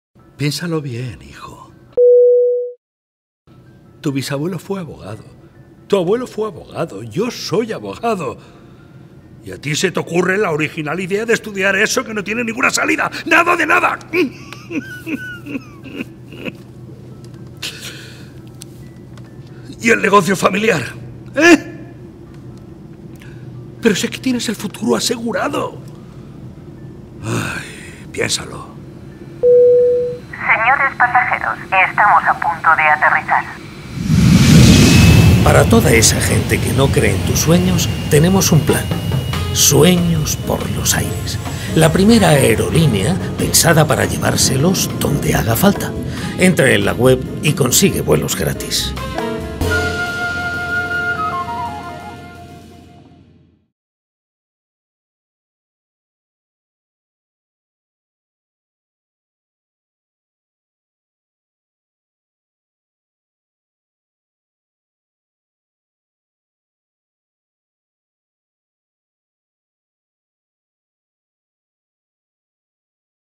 FLORIDA UNIVERSITARIA: Sueños por los aires (Padre cansino)
Convencidos de que nadie puede echar a perder los sueños de los jóvenes, creamos una aerolínea ficticia para Florida Universitaria. El objetivo, en tono de humor y cargado de positividad, era enviar donde haga falta y neutralizar opiniones bienintencionadas que “tiran por los aires” los sueños de los futuros estudiantes con frases como: “no tiene salida” o “sigue con el negocio familiar”.